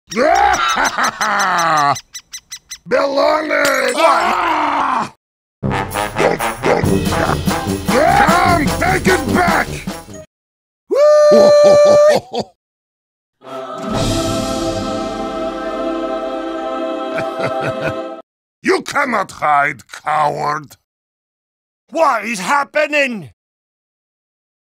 Fat Heavy Vs. Blue Heavy Golden Sandwich Collab
This is my sfm video for the collab hope you like this sh!t post I made in the last two days :)
Edit: owo notices your white line across the screen